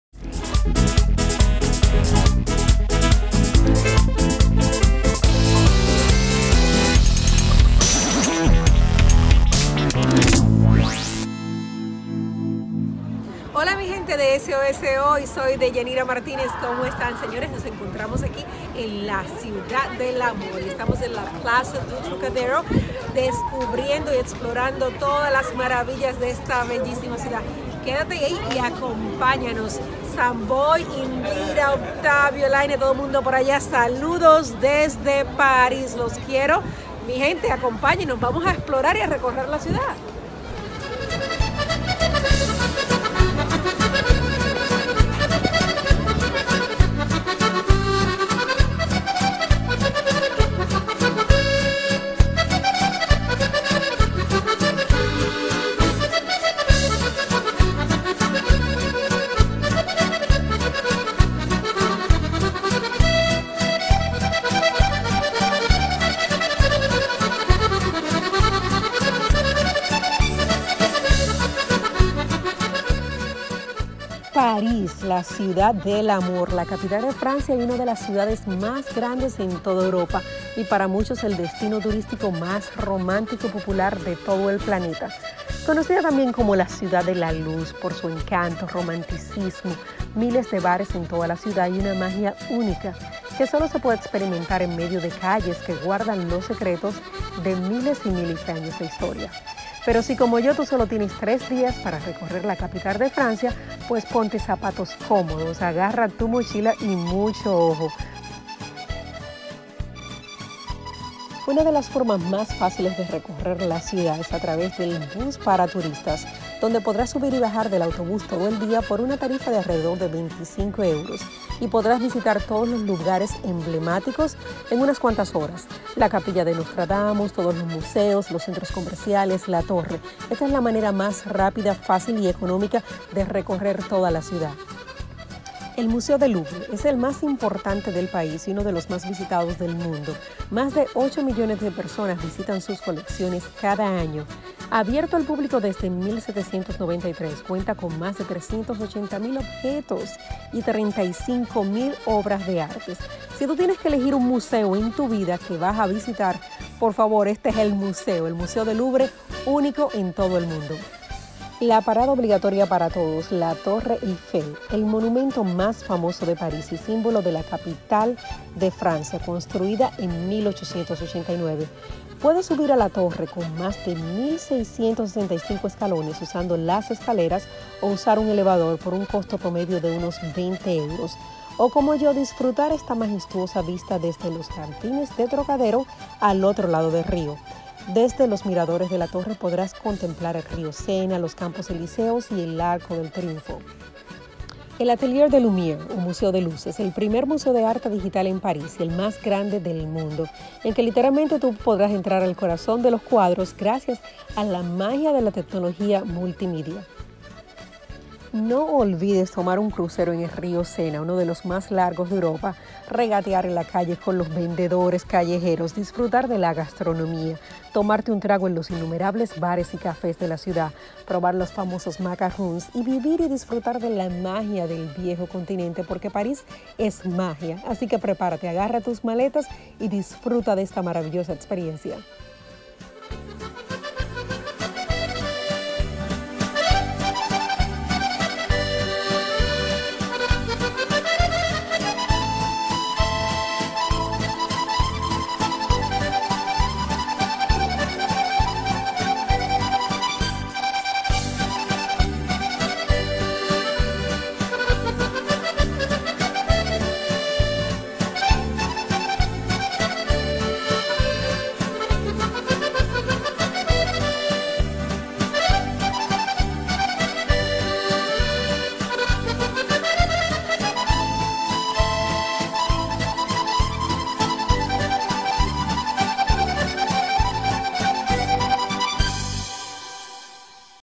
QUE HACER EN PARIS EN TRES DIAS
Recorriendo Paris en 3 dias - Lugares que no debes dejar de visitar en la capital de Francia